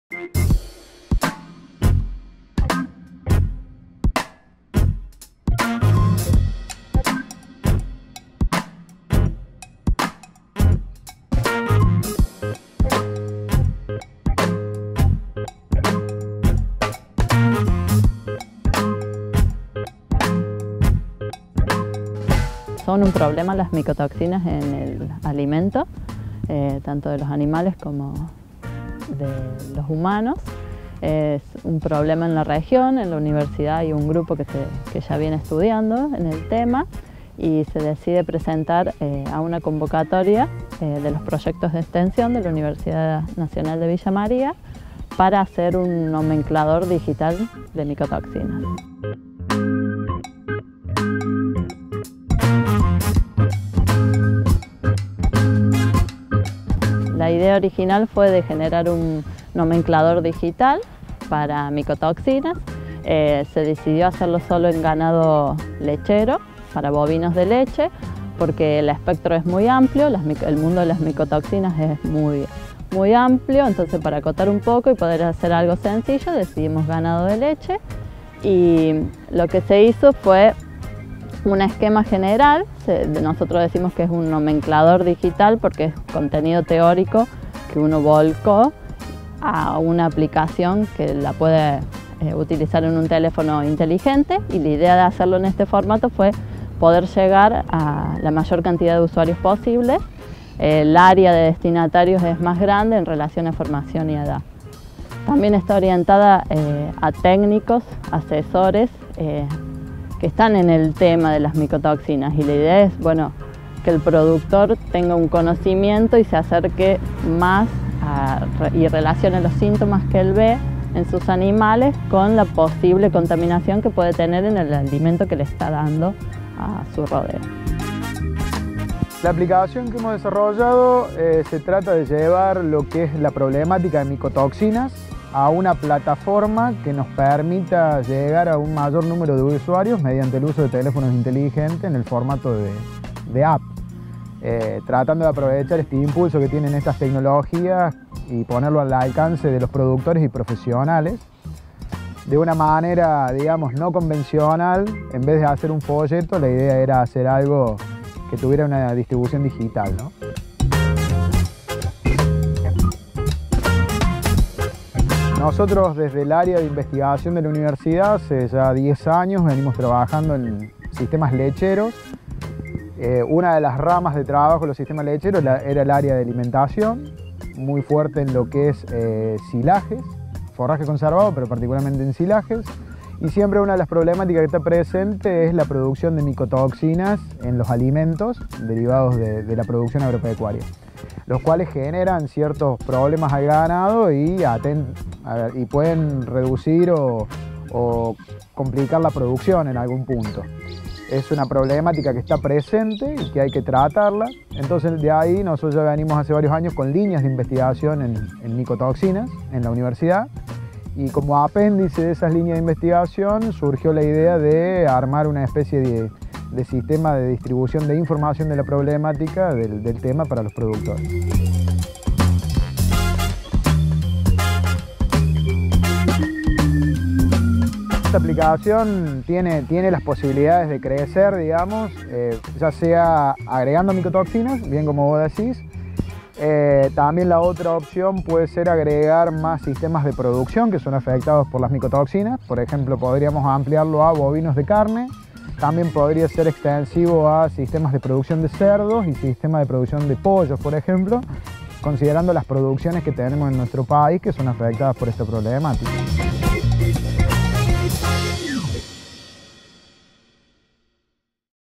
Micotoxinas
En Villa María el INTA y la Universidad Nacional desarrollaron una aplicación para que los productores tengan más información sobre estas sustancias tóxicas.